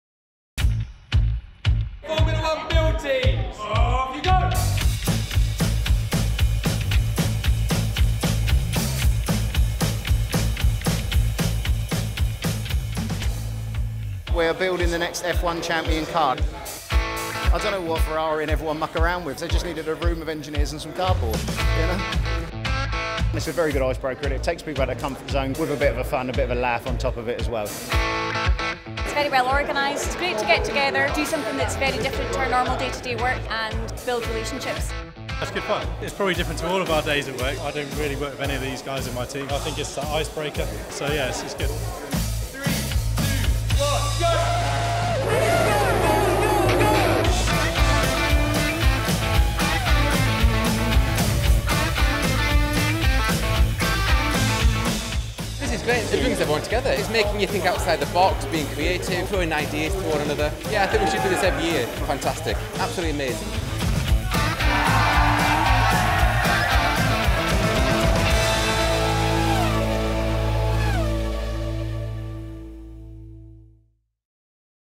EMG Formula One Challenge
This corporate event is almost certainly our staff's favourite. A classic team builder that can't help but put a smile on your face.

Each team is presented with a chassis, which is the starting block for the car; the team must successfully build the car out of the cardboard sheets provided before applying rolls of brightly coloured Fablon to create the colour scheme. Other embellishments will test your creative juices.